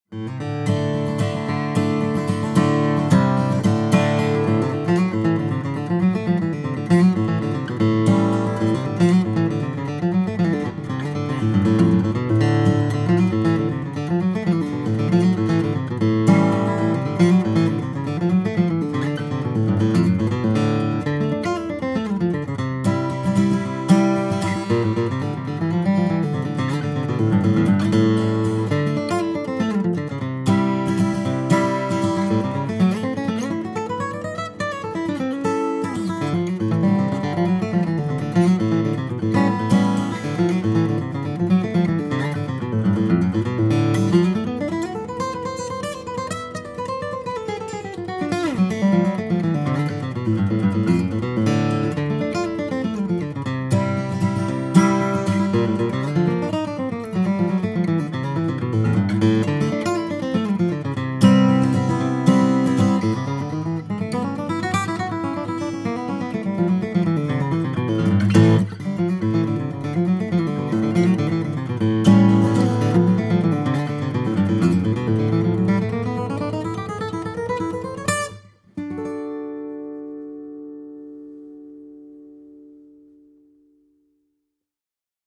Santa Cruz 1934 D Brazilian Rosewood & Adirondack Spruce demo
The now-legendary herringbone dreadnought was first released in 1934, and stands as one of the most highly sought after acoustic guitars of all time, especially among bluegrass flatpickers. This is for good reason, as the combination of forward-shifted scalloped X bracing and Brazilian rosewood made for a powerful bass response and huge volume.

This Santa Cruz dreadnought, inspired by the venerable 1934 original, features Brazilian rosewood back and sides and an Adirondack spruce top with the classic herringbone purfling we all know and love. When flatpicked, this guitar rumbles and roars. The Adirondack top offers projection and brightness, while the Brazilian rosewood lends sweet overtones and complex tonality. As to be expected with these specs, this guitar is a bluegrass monster, packing in many of the features of a pre-war offering, with the stability of a brand new instrument.